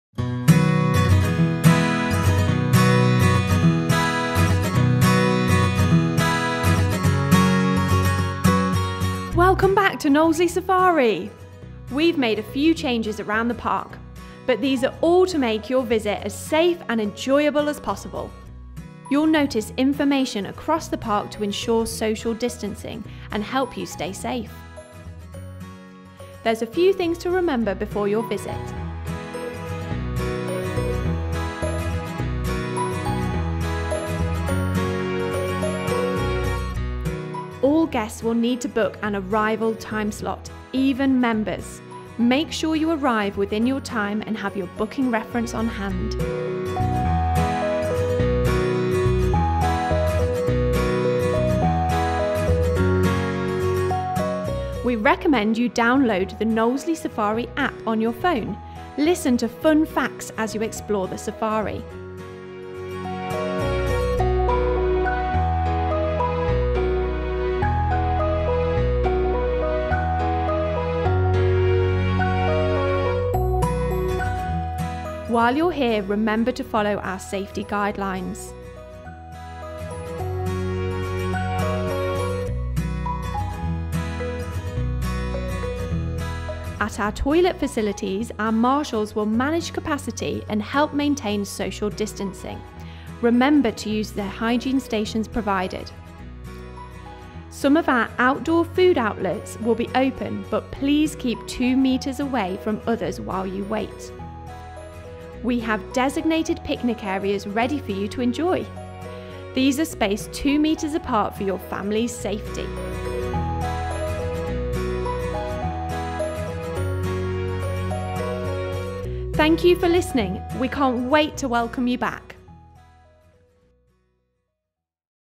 Things you need to know about your COVID safe visit to Knowsley Safari
The safety of our guests and our team is our highest priority as we reopen after lockdown. So we've been working hard to put in extra measures across the safari drive and the foot safari. Follow us as we walk you through the changes you'll see on your day out!

Make sure you've booked online before you visit to help us manage visitor numbers safely!
We cannot allow entry without a booking. Tickets are in high demand right now but we'll be releasing more over the coming weeks.